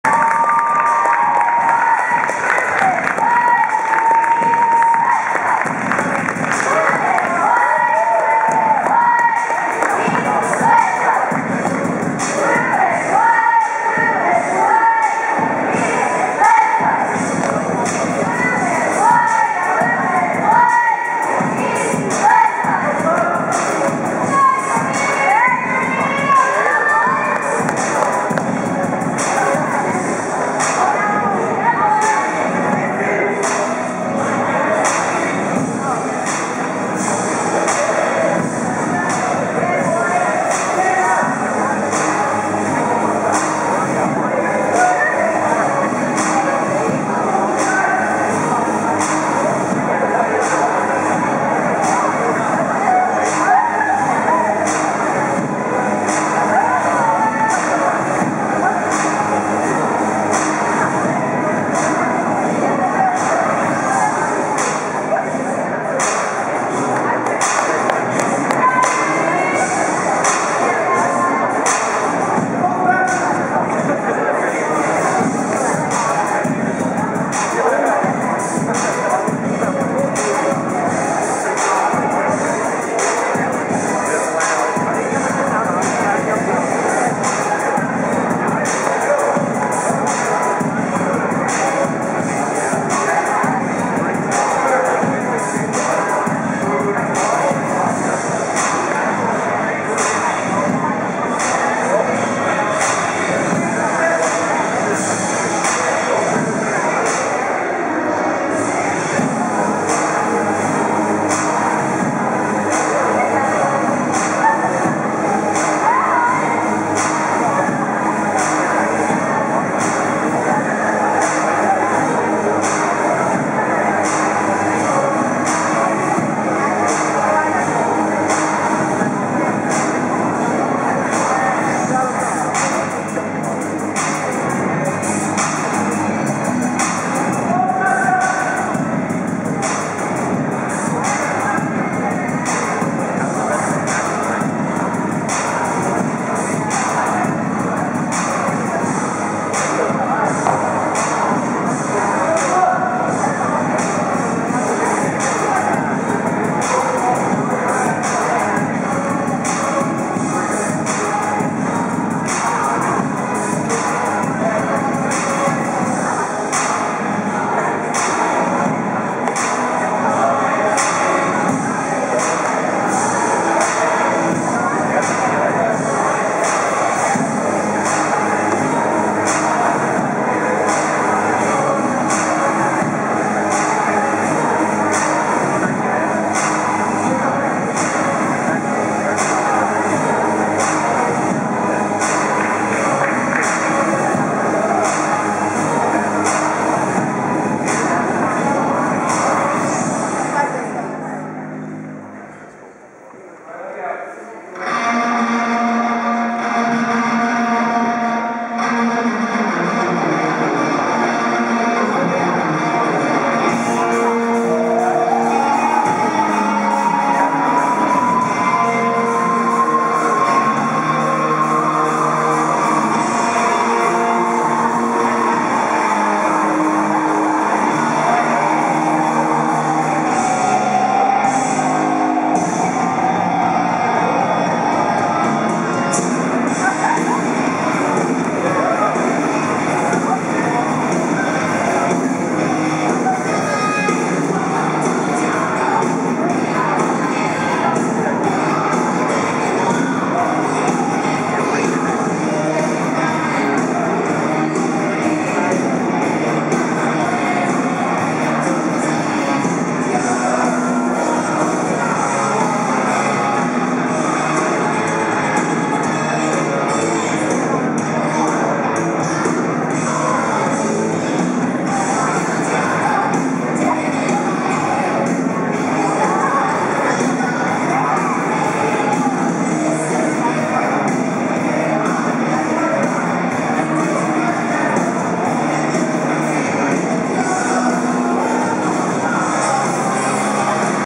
NCSSM v Cedar Ridge HS Wrestling - warm up on Senior Night
NCSSM Men's wresting team warms up before match with Cedar Crest HS.